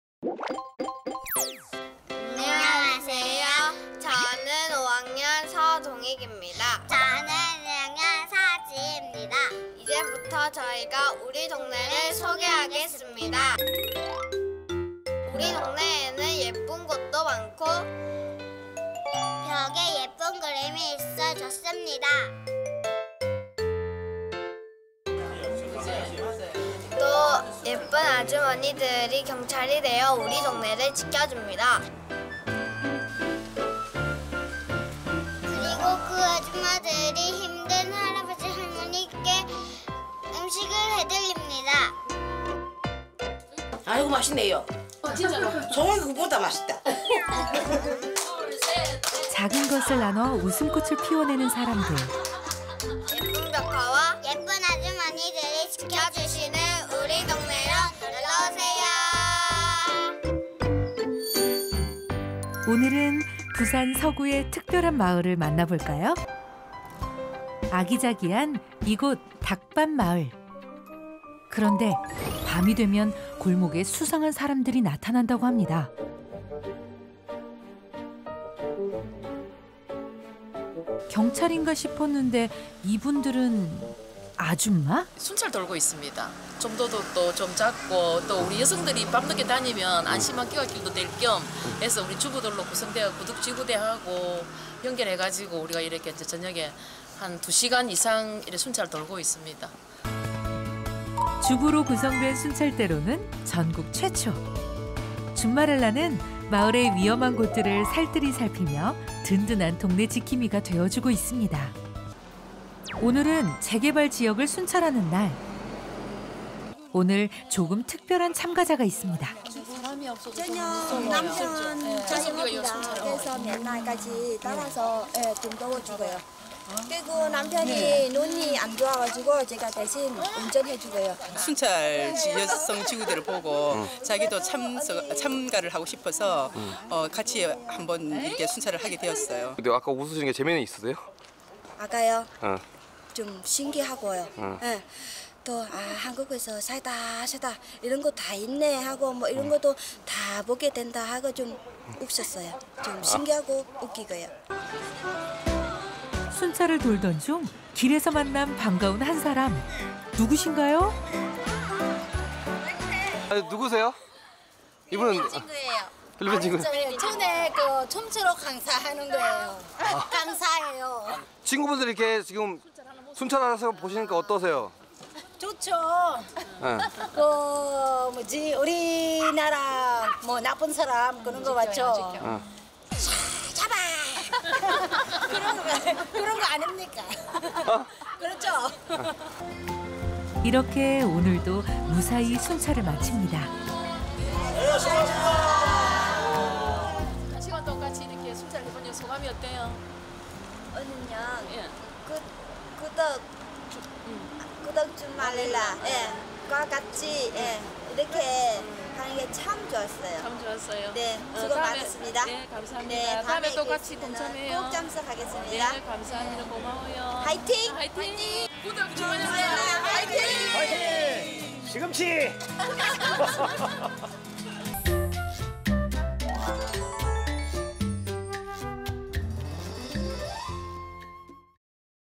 우리 마을 순찰대 줌마렐라! 어머니의 마음으로 마을을 돌보다!
전국 최초로 주부들로 구성된 ‘줌마레라’ 순찰대가 있는 서구 닥밭마을의 이야기!

순찰을 돌며 마을의 어르신들과 안부도 나누고 건강을 살피는 등 따듯하고 든든한 마을 순찰대의 모습이 방송을 통해 소개 될 예정.

 주민들이 모여 사용하지 않는 물품을 자유롭게 판매하는 주민자립형 ‘품마켓’ 행사, 마을 주민 분들의 사랑방이 되어주는 ‘닥밭골 문화 나눔터’의 모습, 동네 어르신 분들과 함께 했던 따듯한 일화들을 만나본다!